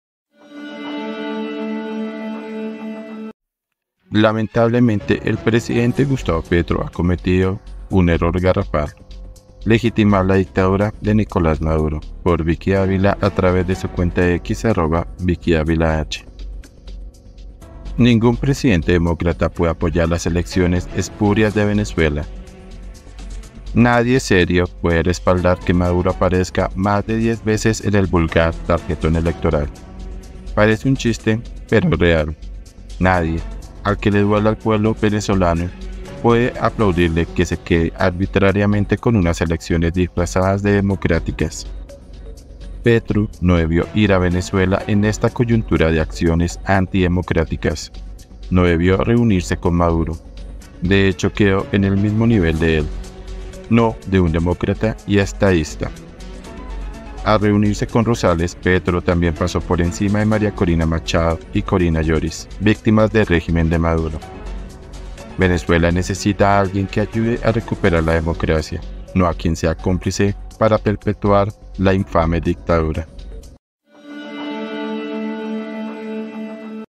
DURO MENSAJE VICKY DÁVILA 📢 "El presidente Gustavo Petro ha cometido un error garrafal"
Ningún presidente demócrata puede apoyar las elecciones espurias de Venezuela. Nadie serio puede respaldar que Maduro aparezca más de 10 veces en el vulgar tarjetón electoral. Parece un chiste, pero es real. Nadie, al que le duela el pueblo venezolanos, puede aplaudirle que se quede arbitrariamente con unas elecciones disfrazadas de democráticas.
VICKY DÁVILA